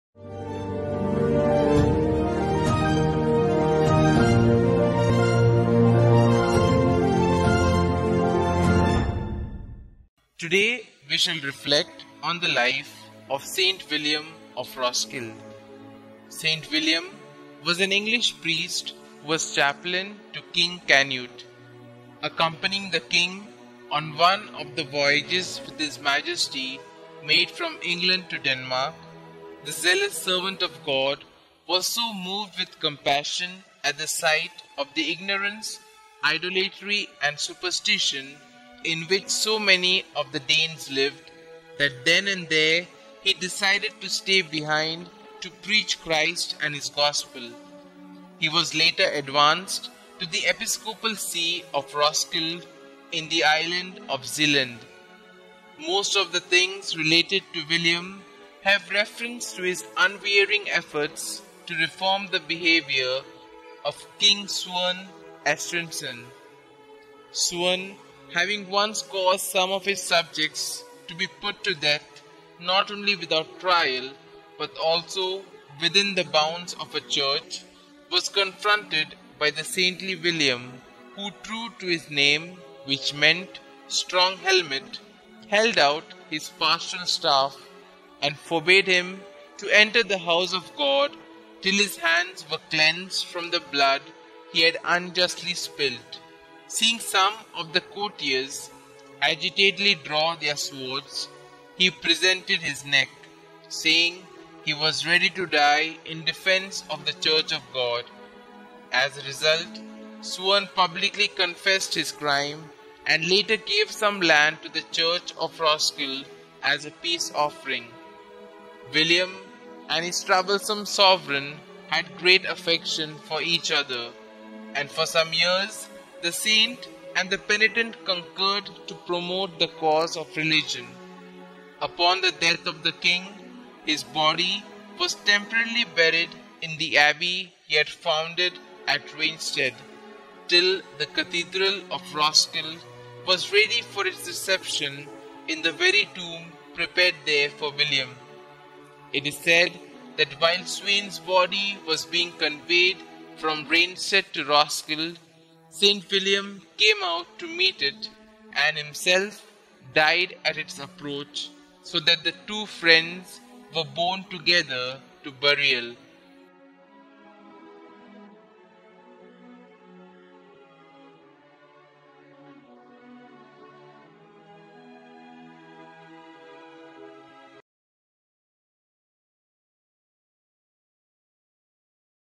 St William Of Roskilde - Saint of the Day with Fr Lindsay - 2 Sept 2022
Here is a brief reflection on the life of St William Of Roskilde  presented by Fr Lindsay Lobo.